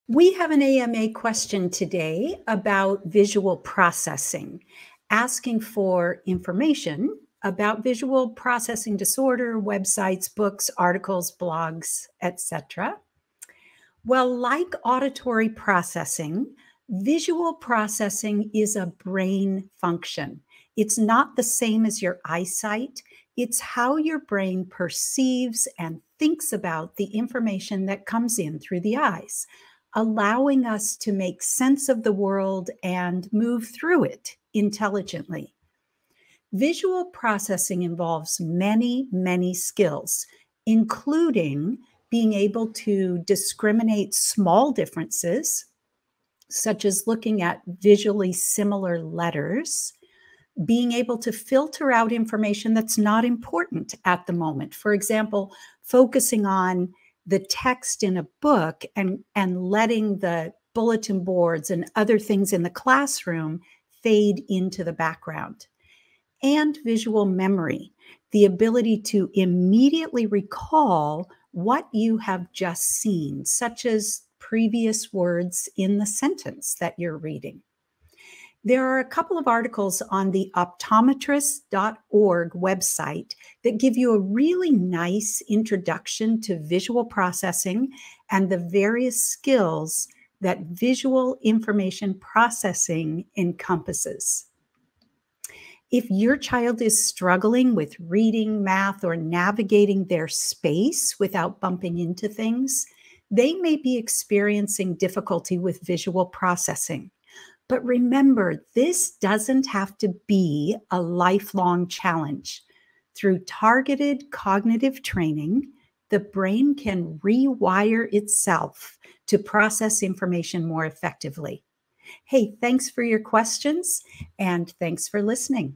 AMA: Visual Processing Disorder
Like auditory processing, visual processing is a brain function.

It is not the same as your eyesight.

It is how your brain perceives and thinks about the information that comes in through the eyes, allowing us to make sense of the world and move through it intelligently.

Articles on the website that give a nice introduction to visual processing and the various skills that Visual Information Processing encompasses.

If your child is struggling with reading, math, or navigating their space without bumping into things, they may be experiencing difficulties with visual processing.

But remember, this does not have to be a life-long challenge! Through targeted cognitive training, the brain can rewire itself to process information more effectively.

➡️ Have a question for Jill?